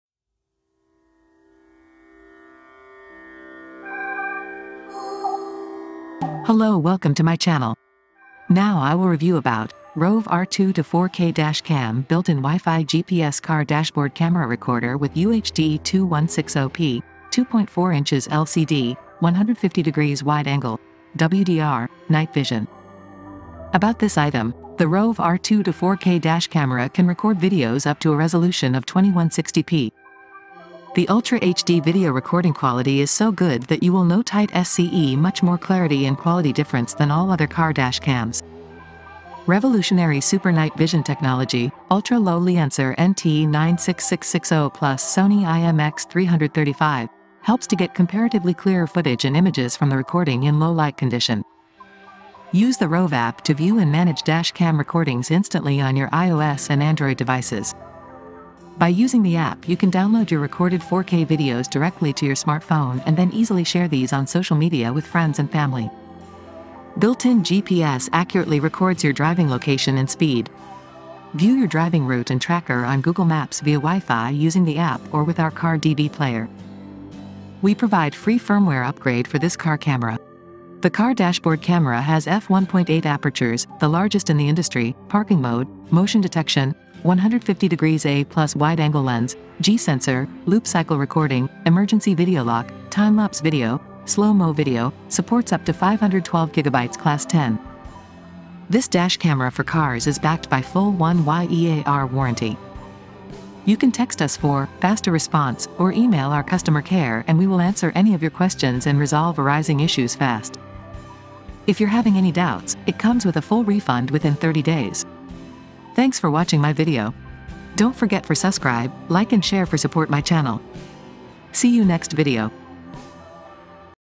Rove R2- 4K Dash Cam Built in WiFi GPS Car Dashboard Camera Recorder with UHD 2160P, 2.4" LCD
Rove R2- 4K Dash Cam Built in WiFi GPS Car Dashboard Camera Recorder with UHD 2160P, 2.4" LCD, 150° Wide Angle, WDR, Night Vision.

🔶 Similar Product :
BRove R3 Dash Cam, 3” IPS Touch Screen, 3 Channel Dash Cam Front and Rear with Cabin, 5.0 GHz WiFi, Built-in GPS

About  :
- The Hardware is meticulously designed to provide crystal clear 4K videos not only during the day but also during the night-time with its ultra-low light Sony STARVIS sensor with super night vision technology.

- The Advanced Software is the heart of this dash camera. We have spent countless hours making its custom-designed software for the user-friendliness and personalized experience that we desire in today’s world.

We provide how-to videos to help you set up your camera easily, personalized customer care and a beautifully written user manual for any dash cam enthusiast.